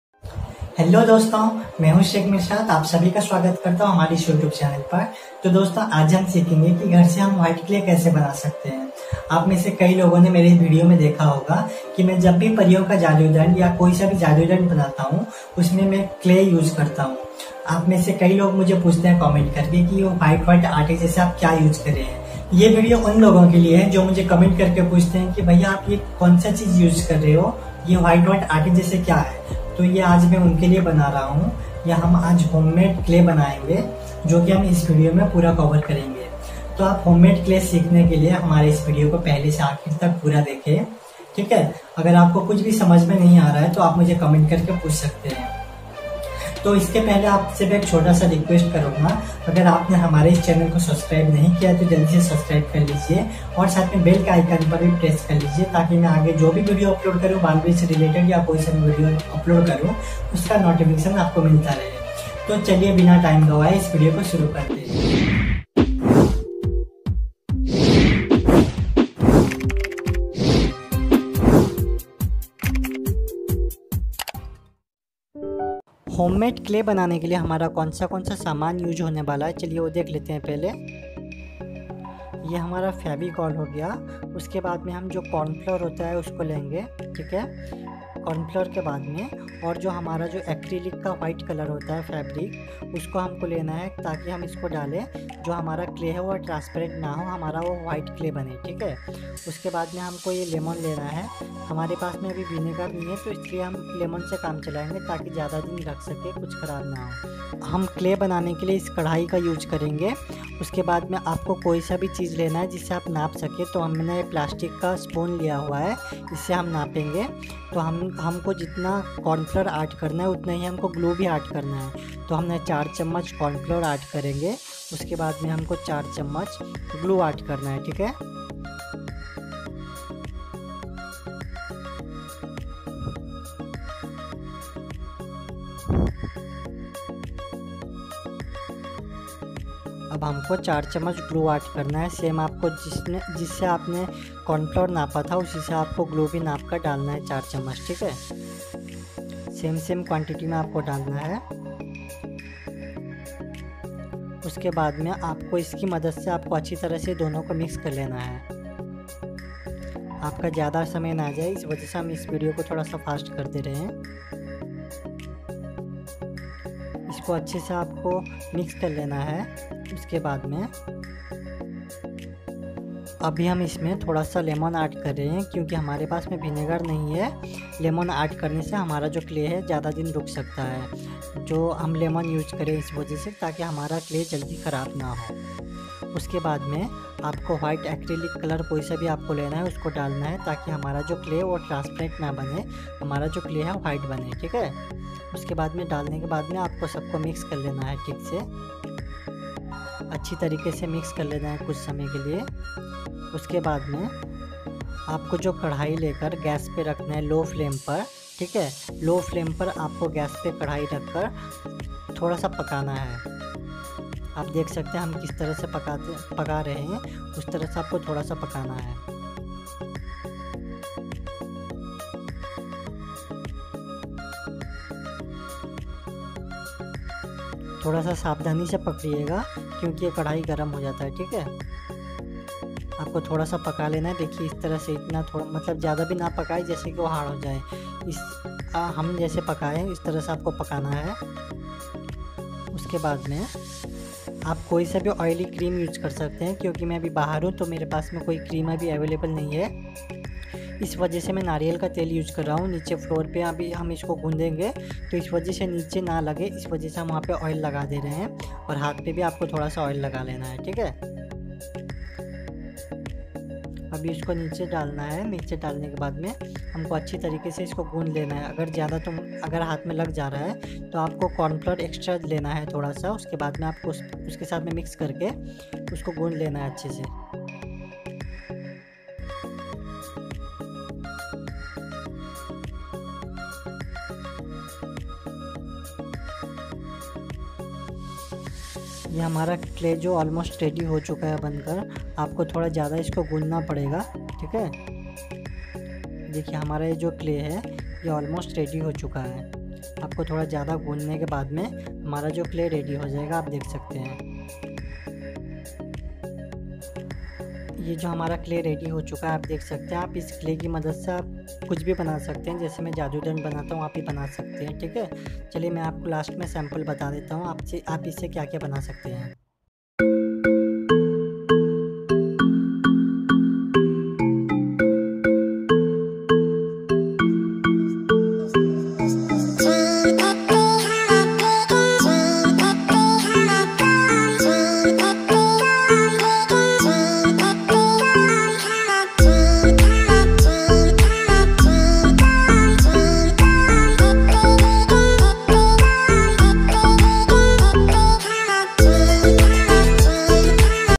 How to Make Art Clay at Home || clay को यूज करके हम जादुई दंड बना सकते हैं || Homemade Clay
Best Homemade Airdry Clay/ Homemade Cold Porcelain Clay On Gas Stove By Saikh Mirsat

This is requested Tutorial on How to make Homemade Airdry clay or cold porcelain clay on Gas stove and Tips to keep in mind while working with this clay. Hope you all will find it helpful

Materials Used: 4 small Cup Cornflour, 4 small Cup Glue, 2 Tablespoon Vinegar or Limejuice, 2 Tablespoon Oil(Any cooking oil or baby oil), 1 Teaspoon White acrylic color